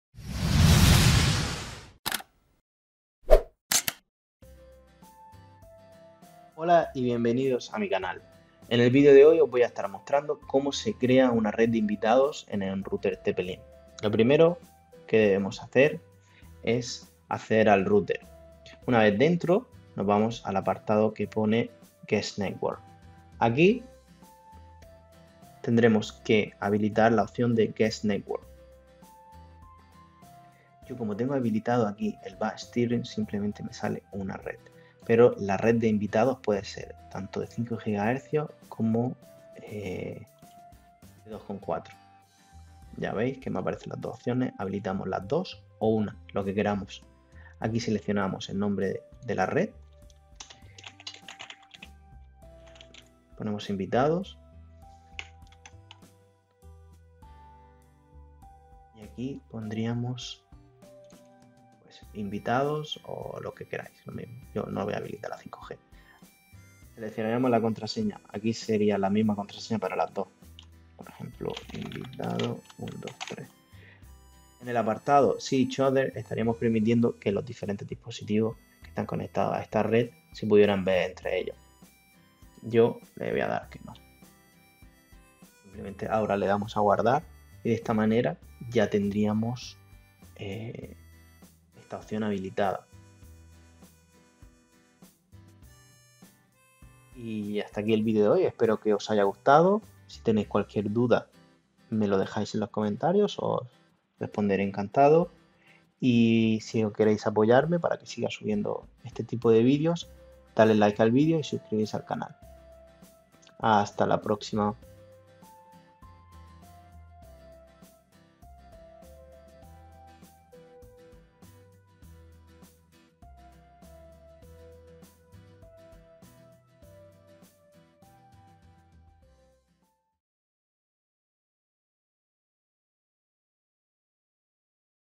✅NO DES TU CLAVE WIFI | COMO CREAR UNA RED DE INVITADOS ROUTER ONT TP LINK XC220-G3v✅
¿Quieres apoyarme para que haga mas videos?

¡Bienvenidos al tutorial de cómo crear una red de invitados en un router TP-Link XC220! En este video, te mostraré los pasos sencillos para configurar una red separada y segura para tus invitados en tu router TP-Link XC220-G3v